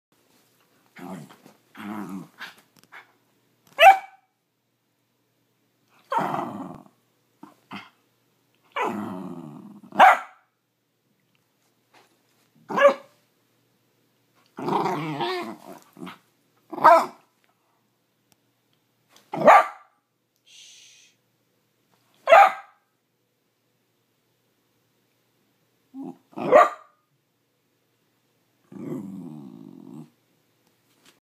Ryder is Hungry
Ryder the dachshund wants me to know he is hungry and wants breakfast